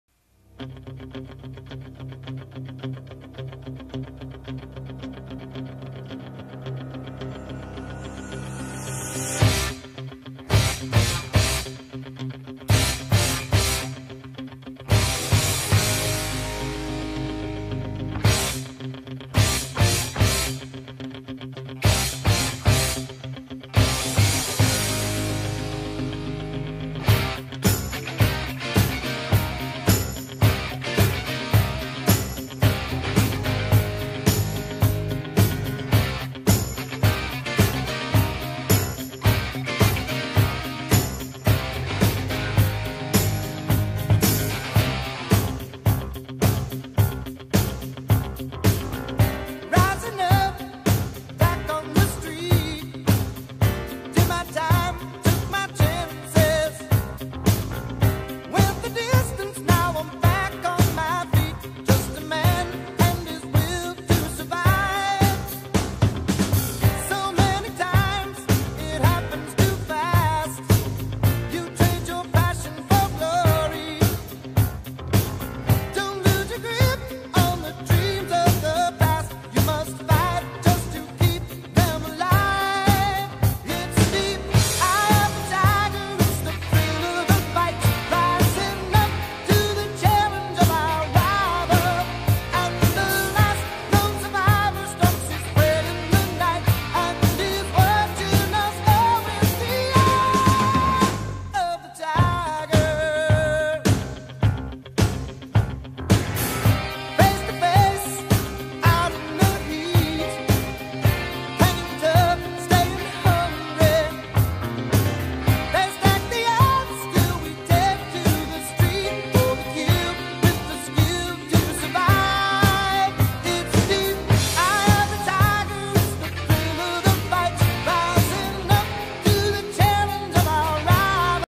Amazing pitbull puppy
Zipper had a wild accident jumping off a deck injuring the growth plates in her elbow beyond repair.  As you can see she's not holding back the love or strength! The 💪